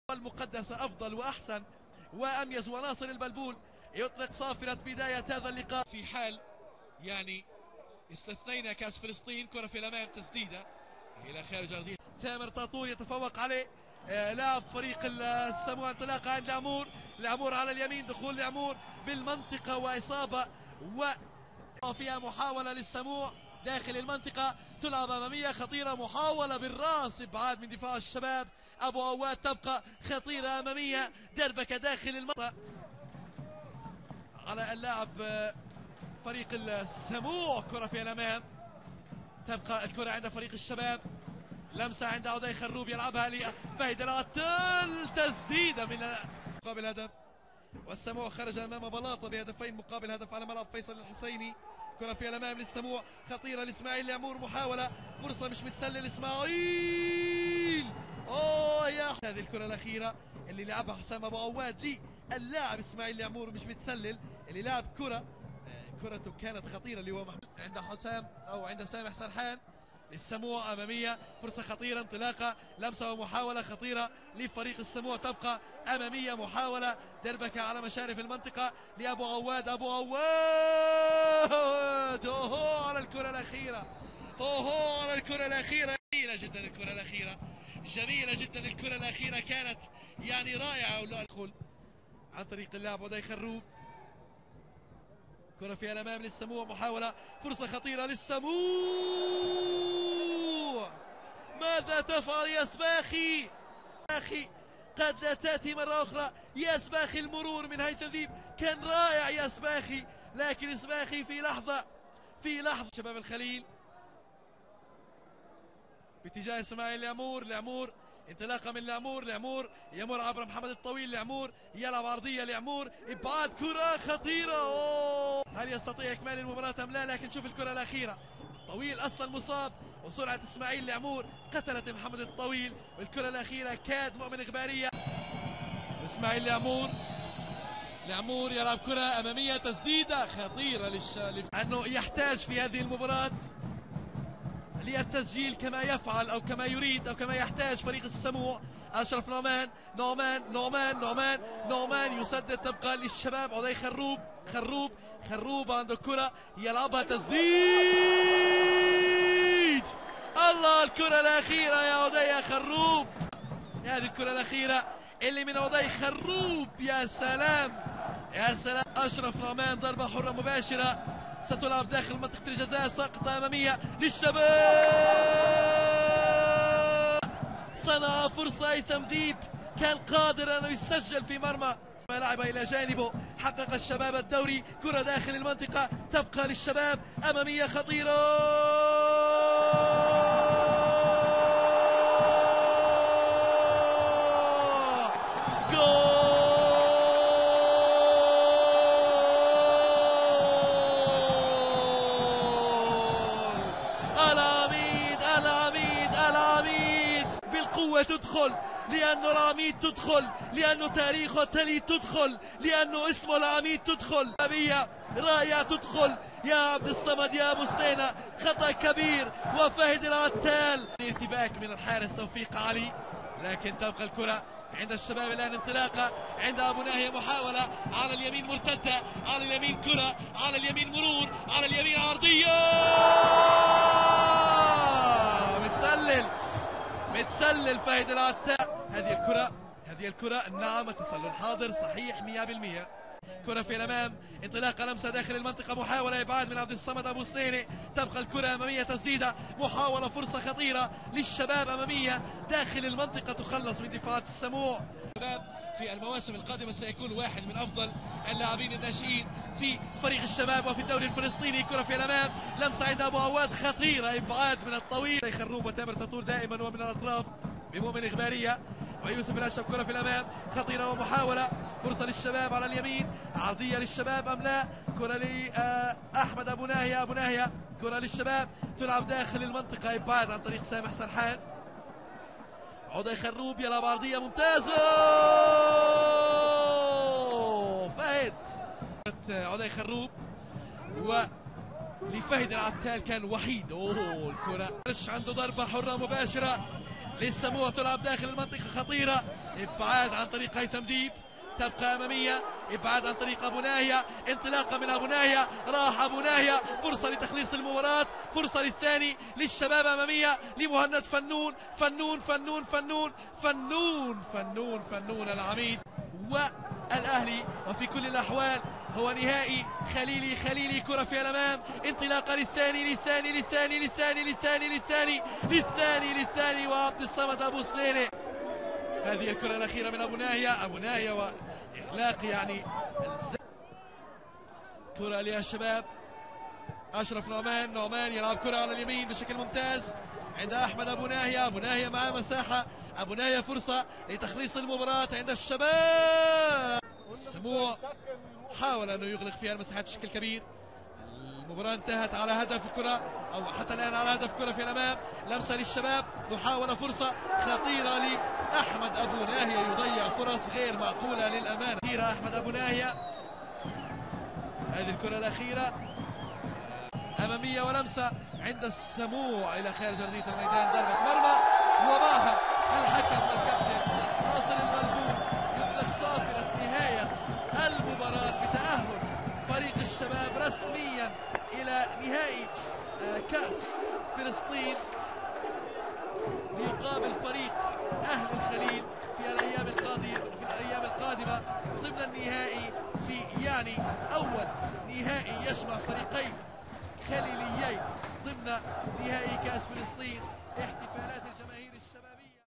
ملخص شباب الخليل 1-0 السموع - نصف نهائي كأس فلسطين 2017
شباب الخليل 1-0 السموع
نصف نهائي كأس فلسطين 2017
سجل لشباب الخليل : فهد عتال
تعليق : مهند قفيشة